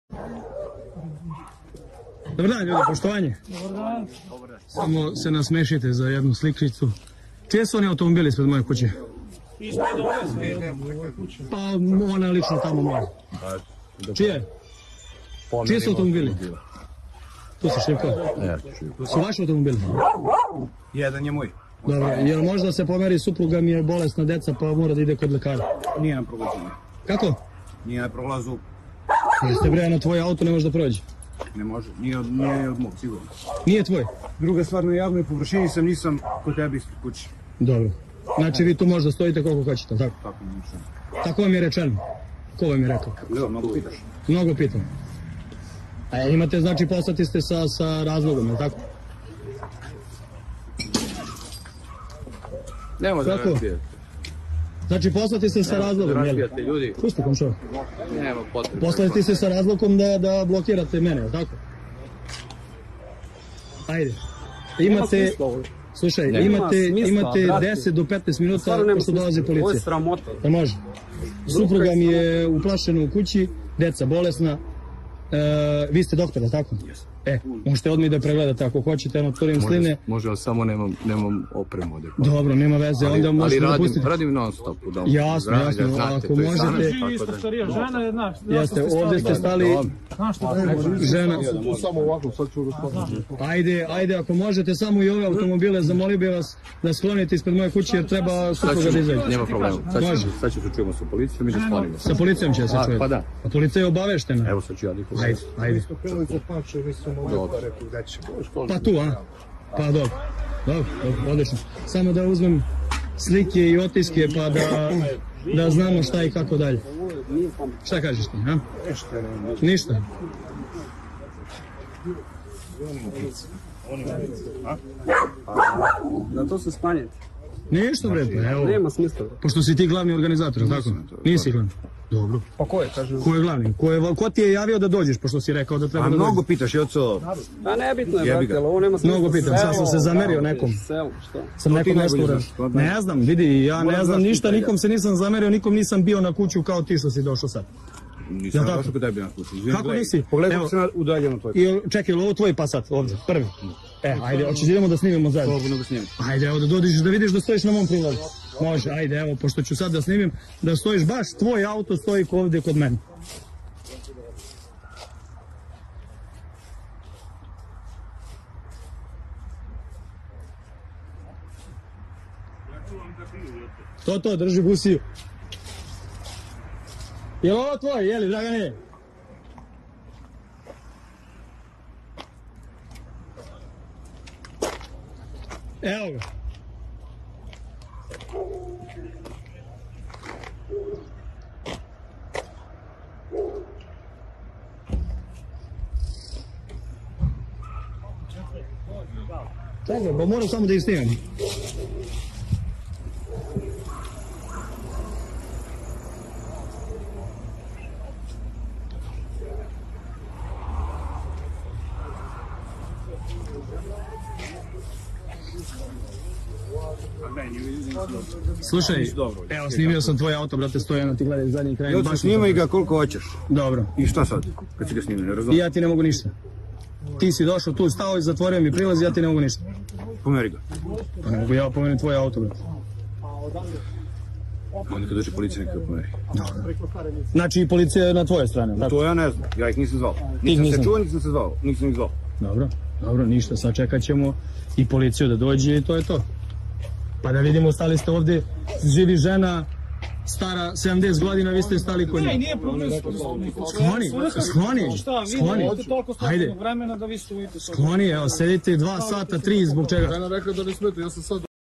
VIDI OVO SRBIJO! BATINAŠI SNS a NAPRAVILI HAOS U BAVANIŠTU!
Regionalna televizija

Poljoprivrednik iz Bavaništa: Grupa predvođena SNS funkcionerom kolima blokirala moju kuću

Poljoprivrednik Jovan Jovanov iz udruženja "Složni Pidikanci" iz Bavaništa, koji su danas blokirali ulaz u Pančevo iz pravca tog sela, kaže da je nekoliko ljudi kolima blokiralo njegovu porodičnu kuću u Bavaništu.

Kako se vidi na snimku koji je dostavio, nekoliko automobila parkirano je ispred njegove kuće gde se trenutno nalaze njegova supruga i deca. Automobili blokiraju izlaz na ulicu, a grupa ljudi okupljena je u bllizini njegove kuće.

On tvrdi da okupljene ispred njegove kuće predvodi Dragan Šljivovački koji je lokalni funkcioner Srpske napredne stranke (SNS).